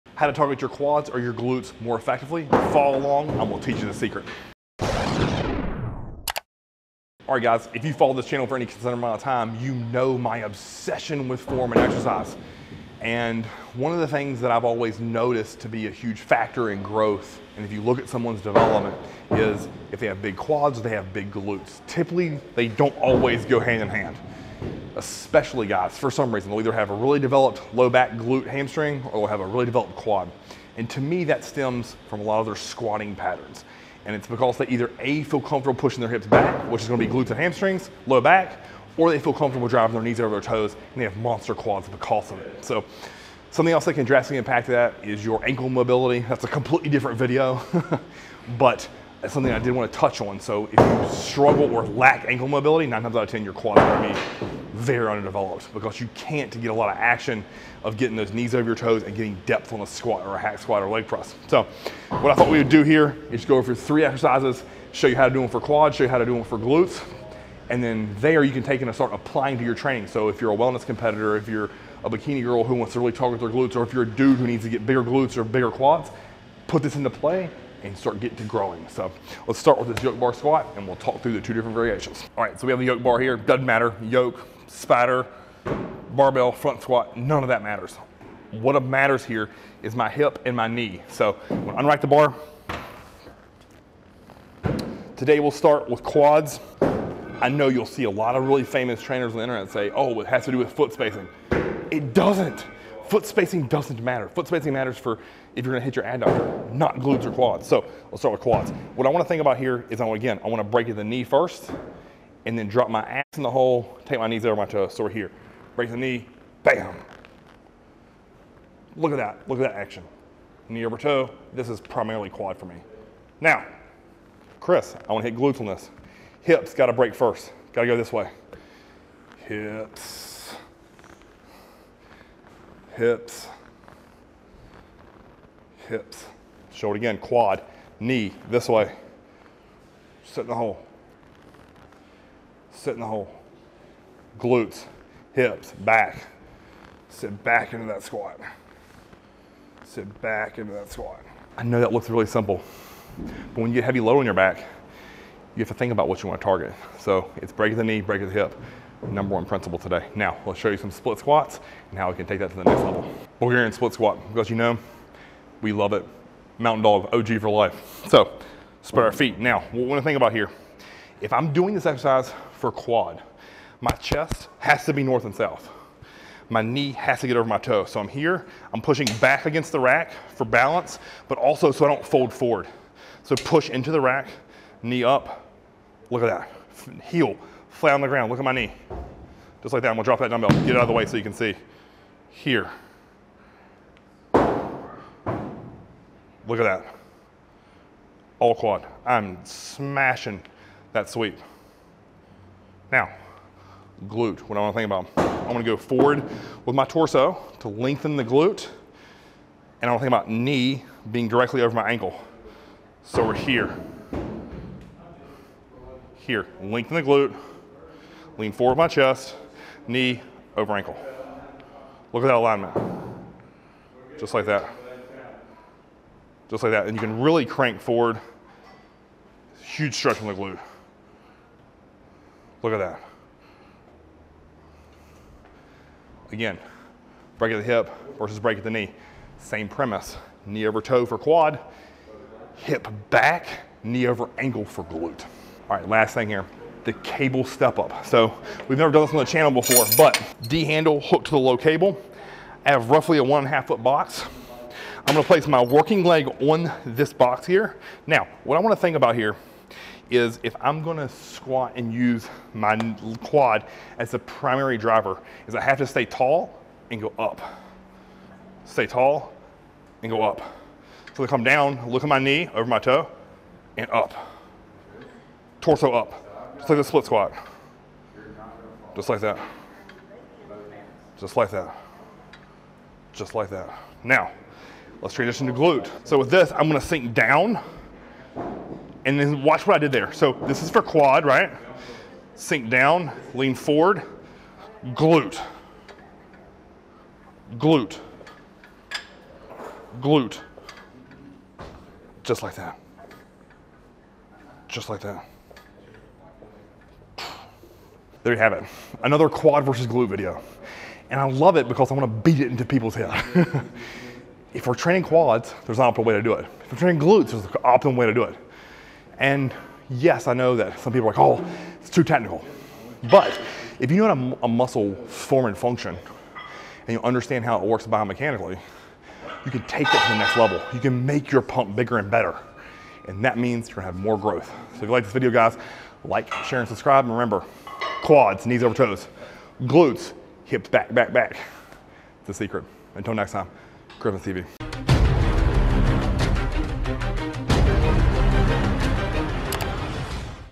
How to Train for Quads OR Glutes (What are the Differences)
Most guys and girls who lift consistently, either have well developed quads or glutes and the reason for that tends to be on their preferred biomechanics. That’s just a fancy way of saying they are stronger at knee flexion (driving their knees over their toes) or sitting back into a lift (great at hip hinging).

In today’s video I go over 3 exercises: squats, Bulgarian split squats and a cable box step up.  With each exercise I show two variations, first quad dominant and second, glute dominant.  So what you need to do is take the muscle group you have lagging and implement that technique to your next lower body day.

Now you won’t be as strong and you may even feel a bit awkward, but this is how you begin to take a weakness and turn it into a strength.

In addition, if you have any follow up questions related to the video, please post them in the comment section, as I’m more than happy to answer it.